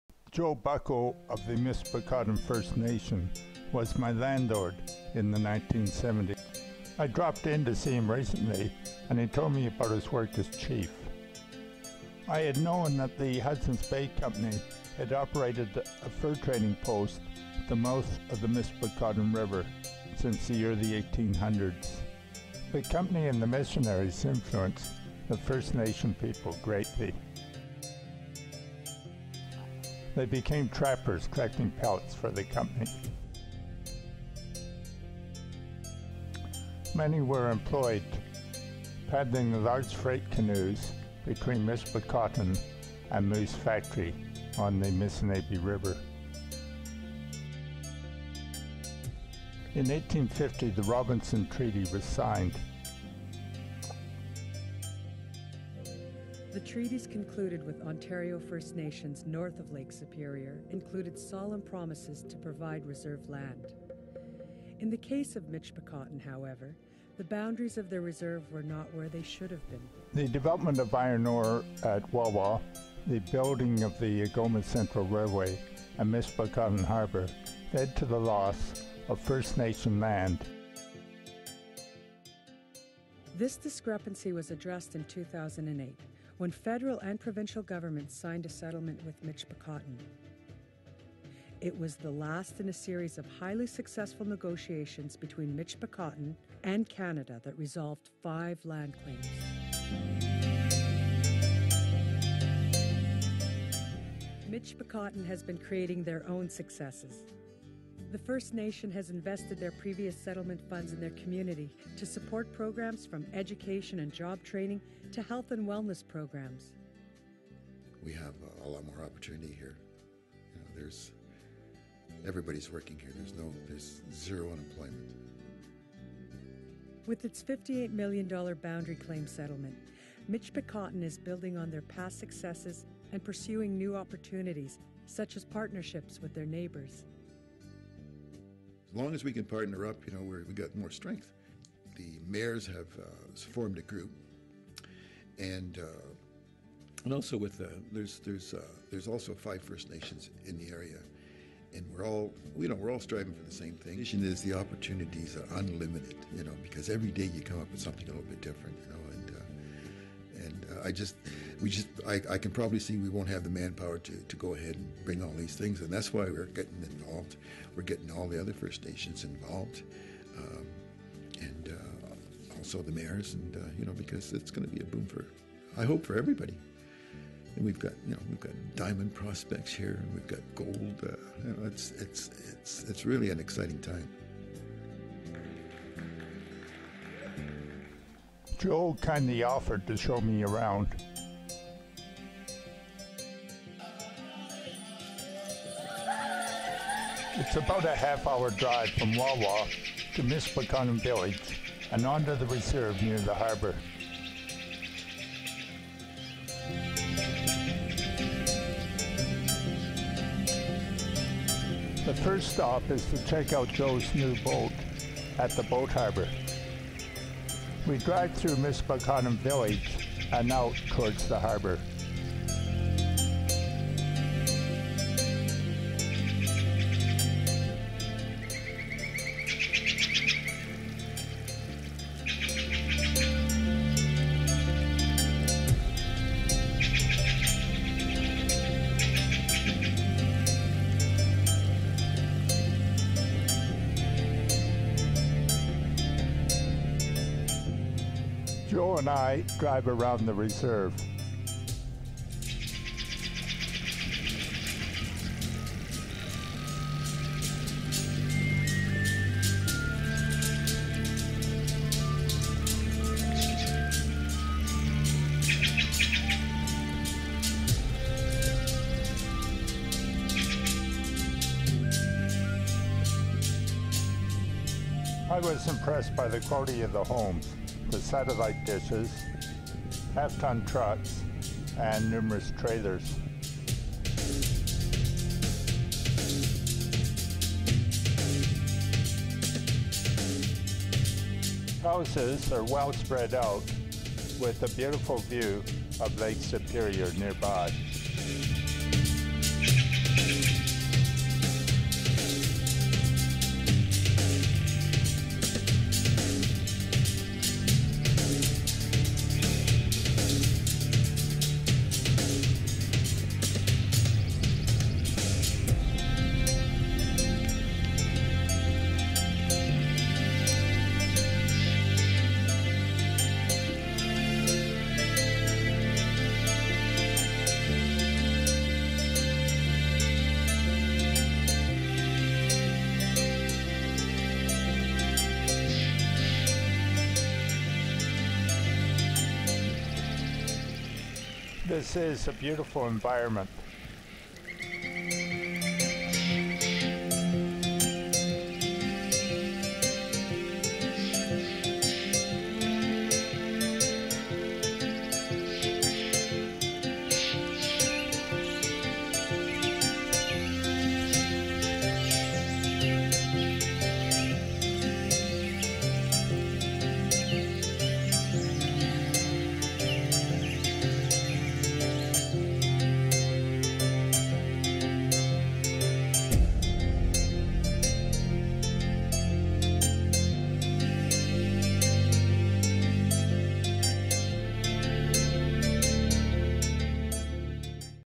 A TOUR OF THE MICHIPICOTEN FIRST NATION RESERVE
GOPRO CAMERAS
Michipicoten First Nation today

Michipicoten First Nation Gros Cap IR49 today and its surrounding lands include extensive coastline along the shores of Lake Superior, the addition of lands settled through various land claim settlements, including the reserves as Missanabie and Chapleau and boasts a pristine and eco rich environment of unparalleled wilderness beauty, unpolluted waters and an abundance of wildlife, birds and indigenous plants. Fishing, hunting and trapping are still practiced by the people and children can be taught the ways of their Ancestors.

Michipicoten First Nation is a vibrant community with approximately 1,020 members dispersed around the globe, building on socio-economic independence and with a strong sense of community and cultural identify, Michipicoten First Nation strives to maintain harmony and balance with Mother Earth, neighboring First Nations and surrounding communities.